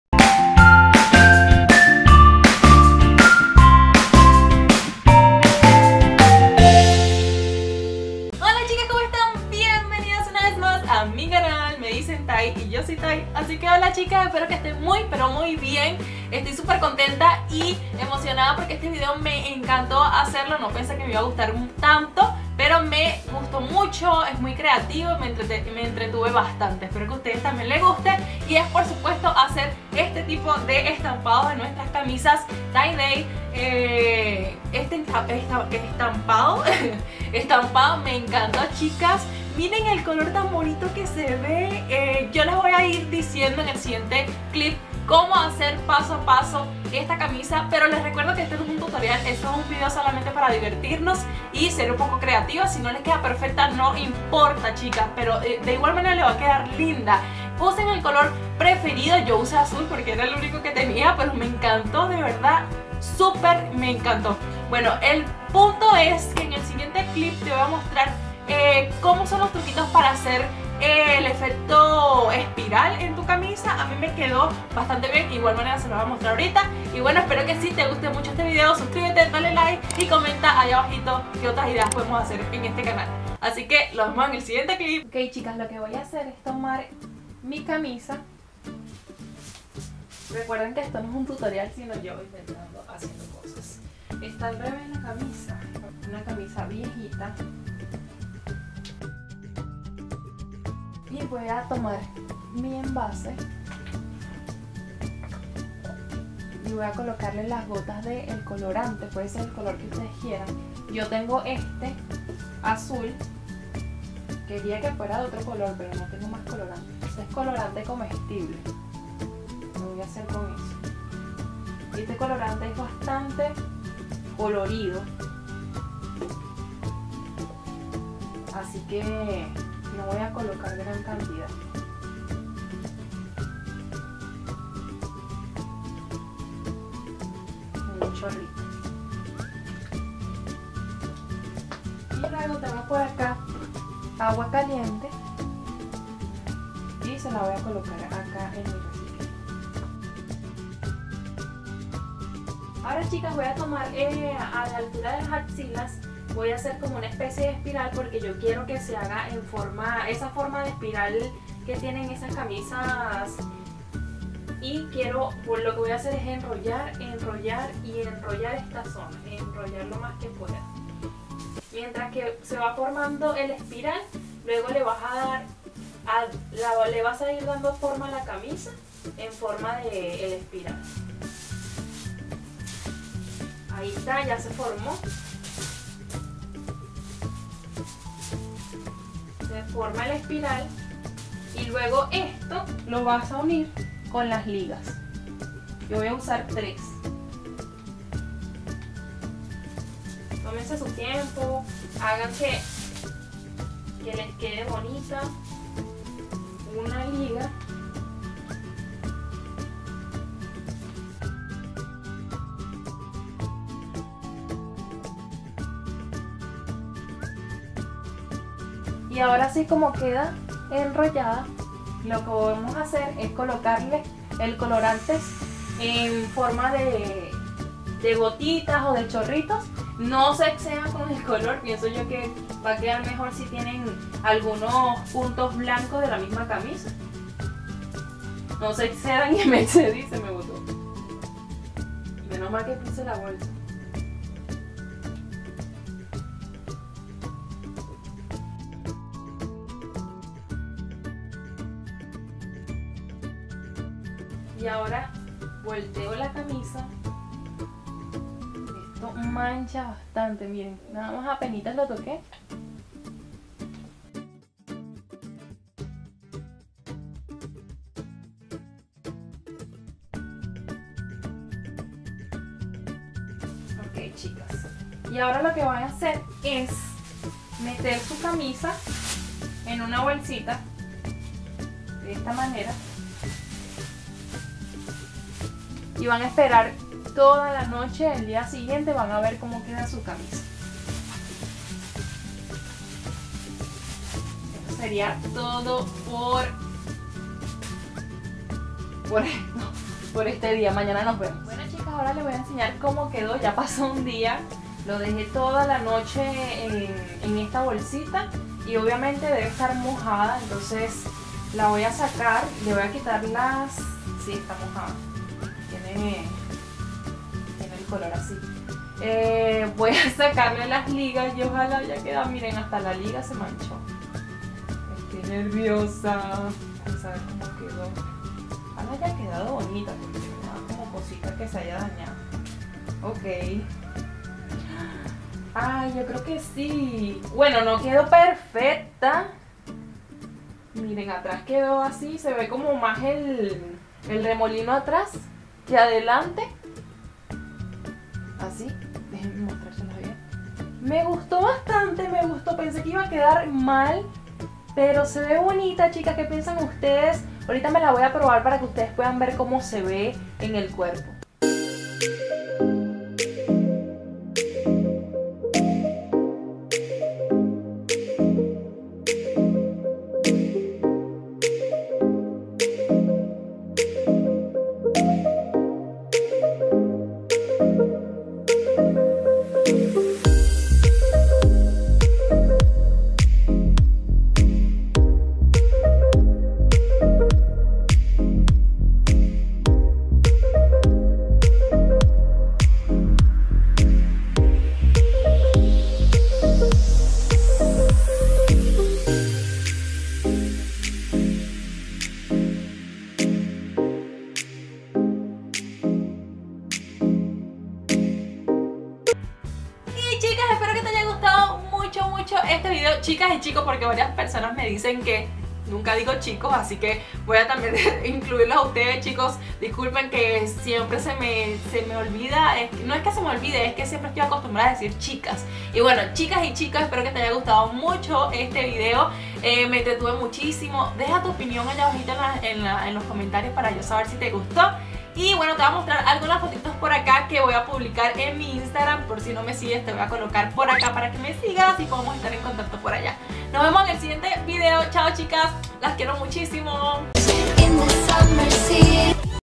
HACIENDO TIE DYE CON COLORANTE COMESTIBLE
hola chicas y chicos! esperoles guste este DYI a mi me encanto hacer este tie day que esta super a la moda ahora !intentalo !espero comentes y si te gustan mis videos les des LIKE Y TE SUSCRIBAS